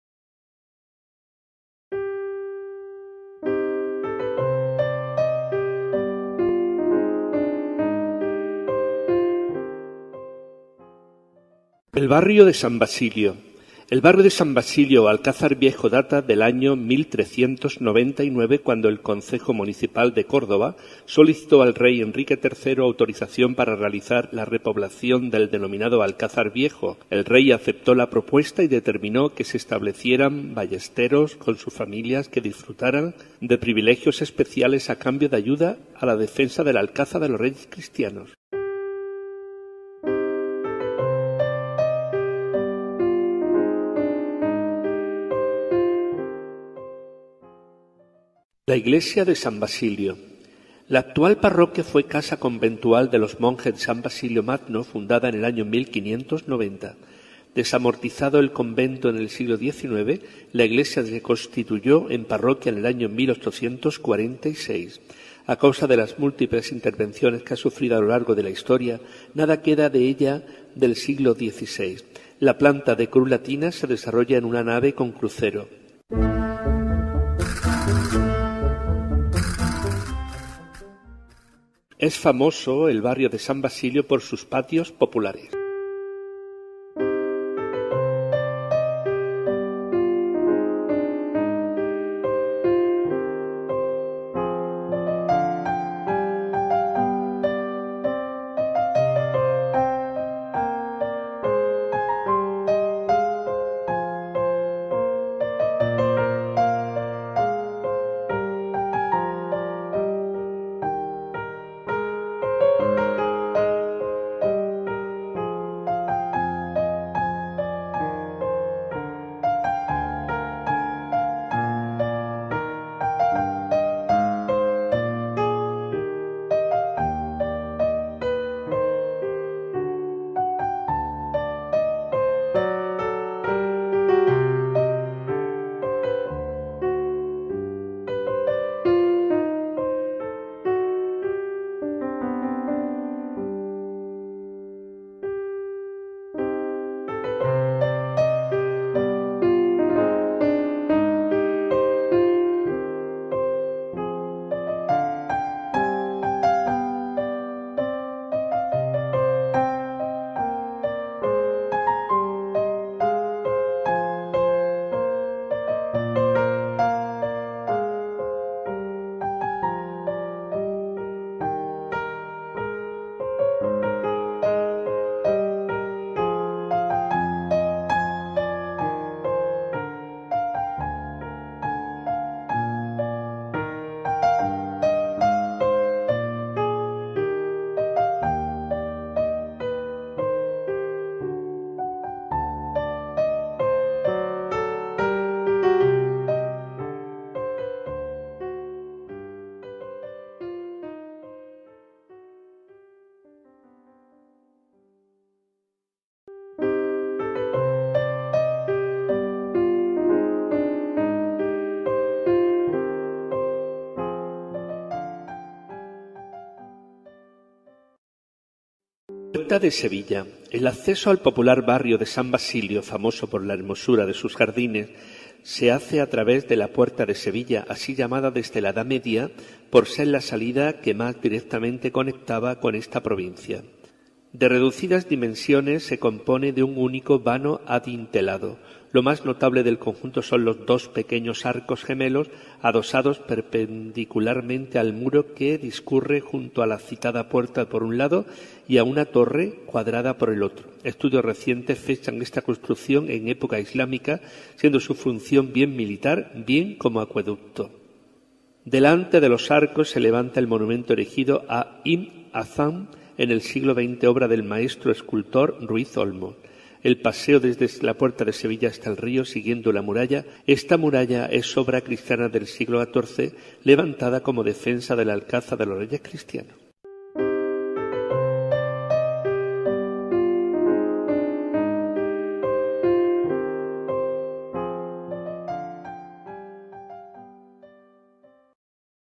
EL BARRIO DE SAN BASILIO Y PUERTA DE SEVILLA EN CÓRDOBA. España.
San Basilio, llamado también, Barrio del Alcázar Viejo,  es un barrio de la ciudad de Córdoba (España), perteneciente al distrito Centro.
Está situado en la ribera del Guadalquivir, en el extremo suroeste del distrito. Famoso por sus patios y cargado de gran historia
   Al Barrio de San Basilio se entra a través de la Puerta de Sevilla, así llamada desde la Edad Media por ser la salida que más directamente conectada con la provincia de Sevilla.